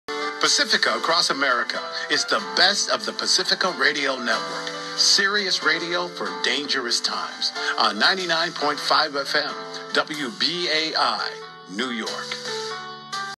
WBAI 99.5 FM New York, NY
Here is the updated top of the hour station ID for WBAI 99.5 FM New York, New York.  On Monday, October 7, 2019 Pacifica Foundation eliminated all local programming from WBAI and is now using a network source called Pacifca Across America.  However, this may not last much longer as WBAI staff were granted a Temporary Restraining Order to return to the station.  Recorded on Tuesday, October 8, 2019 at around 12:00pm EDT.